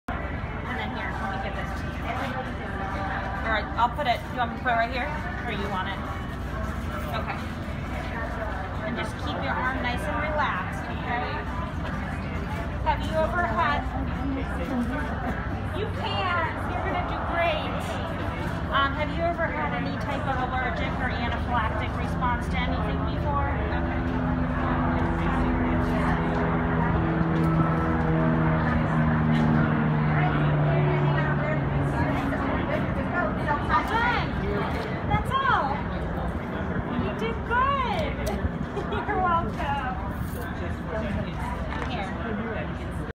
This Senior is Getting a Covid 19 Booster Shot
This senior is getting a covid booster shot to help reduce her chances of a covid infection. Senior citizens are among those who are most vulnerable to covid infections and their effects. It is important for seniors to get vaccinated as soon as possible in order to ensure their safety during this pandemic. As the world continues to grapple with the Covid-19 pandemic, seniors are among the most vulnerable to its impacts. It is essential that they get vaccinated as soon as possible. Vaccines can help protect seniors from serious complications due to the virus.  It helps them stay healthy.

Ad

Burn fat fast using a simple 20-second Japanese tonic.

Ad

The booster shot is a remarkable achievement in the fight against the virus. It has been proven to be effective in providing immunity against the virus,  It can help reduce the risk of infection for seniors. This breakthrough vaccine technology has brought much-needed hope to an otherwise bleak situation, allowing us to protect our vulnerable populations. It is important for them to get vaccinated as soon as possible in order to ensure their safety and well-being. It helps the stay safe and healthy during these difficult times.

More AFSB Shorts